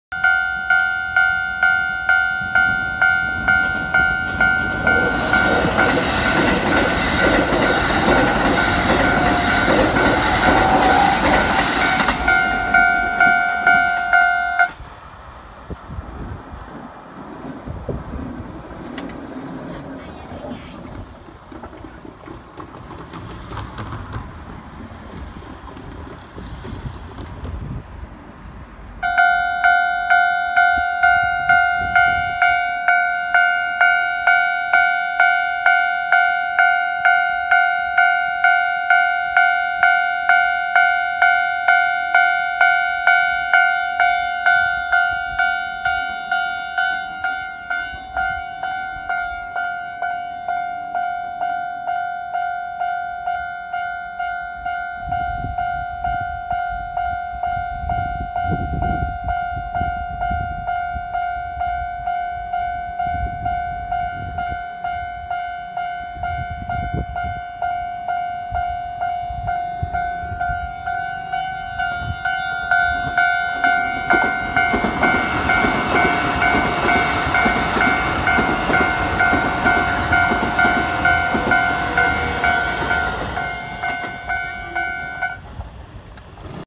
京急線上り8両編成快特青砥行き1000形152～145編成と下り8両編成15D京急沿線の風景ギャラリー号エアポート急行新逗子行キ1000形12019～1208編成が、金沢八景駅～金沢文庫駅間を走行中！
京急線金沢八景駅手前の金沢文庫第2踏切から撮影！
金沢八景駅を発車して、1駅先の次の停車駅の金沢文庫駅へと走行中の、上り8両編成快特青砥行き1000形152～145編成ステンレス車両の電車が、ジョイント音をたてながら金沢文庫第2踏切を通過して行きます！
踏切の警報機が直ぐに鳴り始めます！
車庫車止めに停車中の、6両編成65普通新逗子行き1500形1581、1584編成と8両編成エアポート急行新逗子行き1000形177～184編成の電車！！
金沢文庫駅方向から、下り8両編成15D京急沿線の風景ギャラリー号エアポート急行新逗子行き1000形12019～1208編成けいきゅんの電車が、金沢文庫第2踏切をジョイント音をたてながら通過して、1駅先の次の停車駅の金沢八景駅方面へと走行中です！